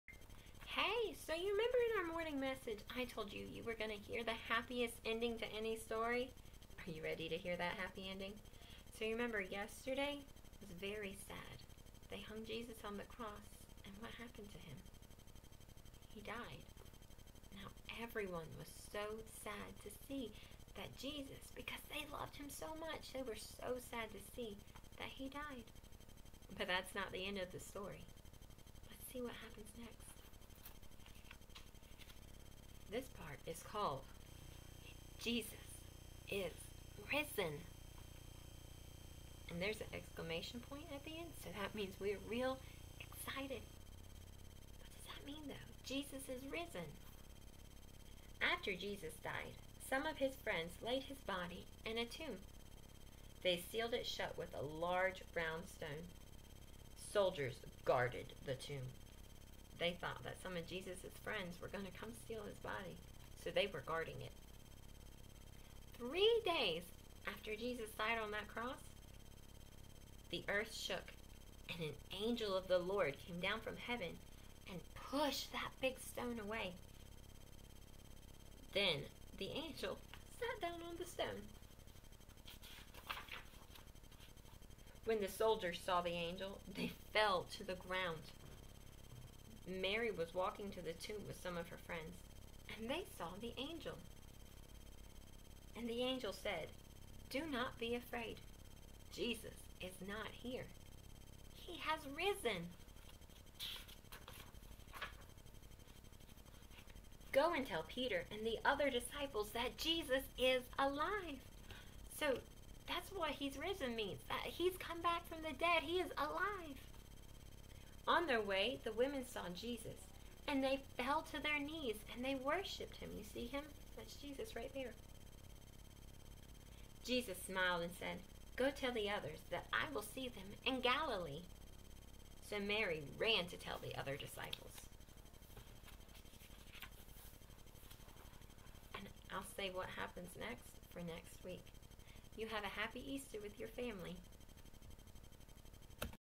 Bible Story 4/9
The Last Supper and Jesus is Arrested and Crucified